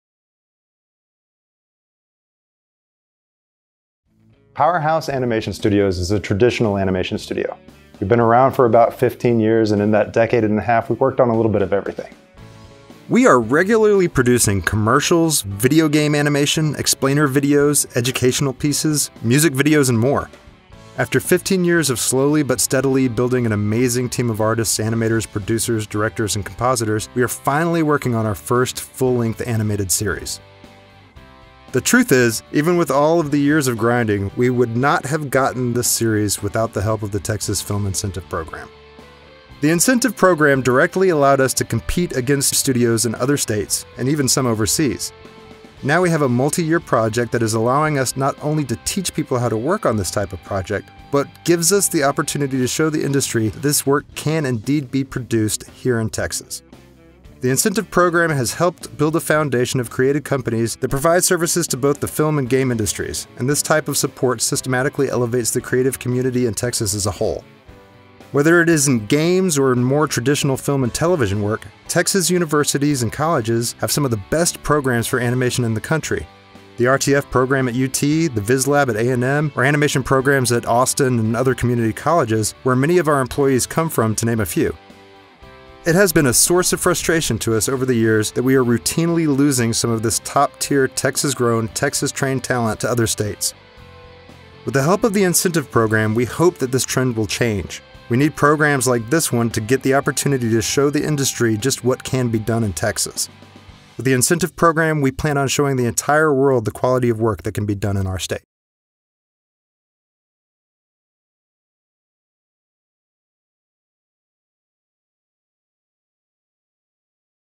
TXMPA Powerhouse Animation
See how the Texas Moving Image Incentive Program directly effects Texas creatives.